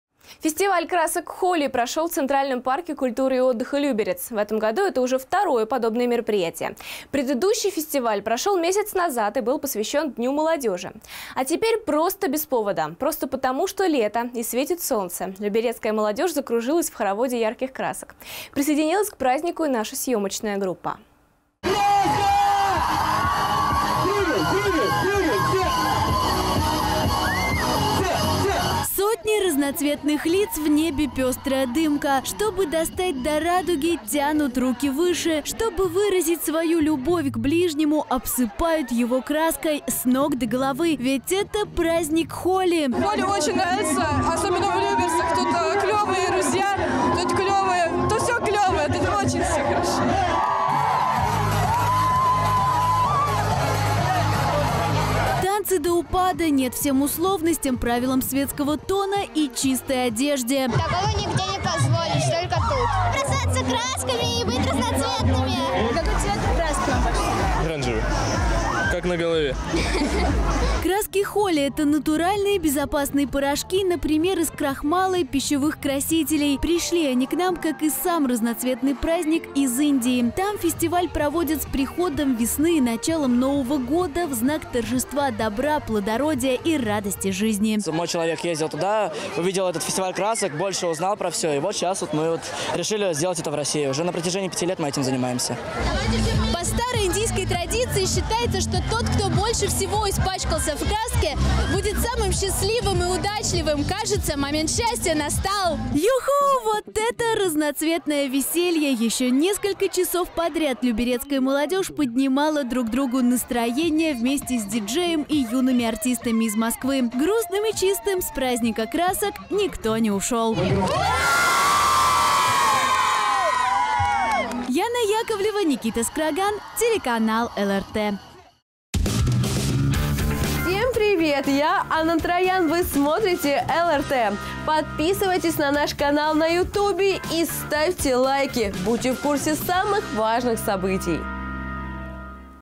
Фестиваль красок Холи прошел в Люберцах
Фестиваль красок Холи  прошел  в Центральном  парке культуры и отдыха Люберец. В этом году это уже второе подобное мероприятие. Предыдущий фестиваль прошел месяц назад и был посвящен Дню молодежи. А теперь – просто без повода, просто потому, что лето и светит солнце, люберецкая молодежь закружилась в хороводе ярких красок.

Сотни разноцветных лиц! В небе пестрая дымка. Чтобы достать до радуги - тянут руки выше! Чтобы выразить свою любовь к ближнему – обсыпают его краской. С ног до головы! Ведь это праздник Холи!

Танцы до упаду, НЕТ - всем условностям, правилам светского тона и чистой одежде!

Краски Холи – это натуральные безопасные порошки, например, из крахмала и пищевых красителей. Пришли они к нам, как и сам разноцветный праздник из Индии. Там фестиваль проводят с приходом весны и началом нового года, в знак торжества добра, плодородия и радости жизни.

Василий Чуриков, организатор фестиваля красок Холи:

«Мой человек ездил туда. Увидел этот фестиваль красок. Больше узнал про все. И вот сейчас мы решили сделать это в России. Уже на протяжении 5 лет мы этим занимаемся».

По старой индийской традиции считается, что тот, кто больше всего испачкался в краске, будет самым счастливым и удачливым. Кажется, момент счастья настал.
Вот это разноцветное веселье! Еще несколько часов подряд люберецкая молодежь поднимала друг ругу настроение вместе с ди-джеем и юными артистами из Москвы.  Грустным и чистым с праздника красок - никто не ушел.

Яна Яковлева, Никита Скраган